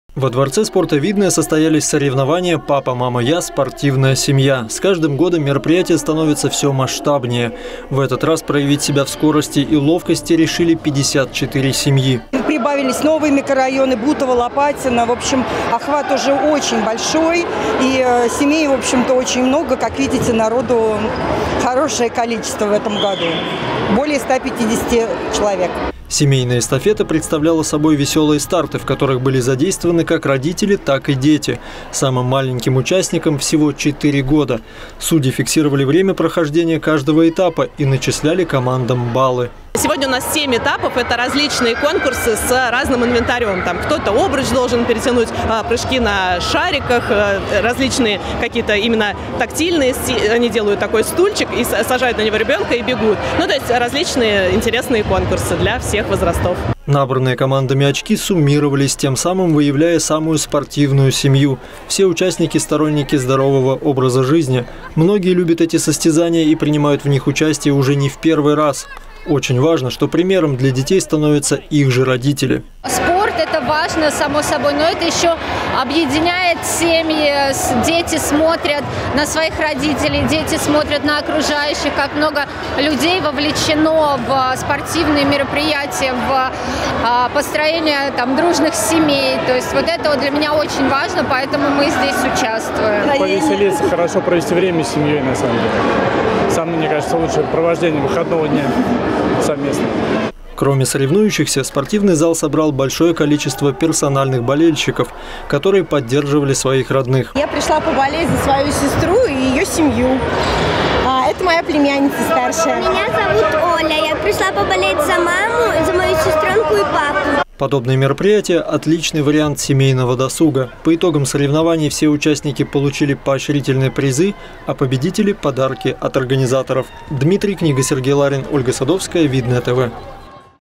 СПОРТИВНАЯ СЕМЬЯ
Традиционные семейные соревнования «Папа, мама, я - спортивная семья» уже больше 10 лет проводятся в нашем муниципалитете. Все участники заряжаются хорошим настроением, а с каждым годом их становится всё больше.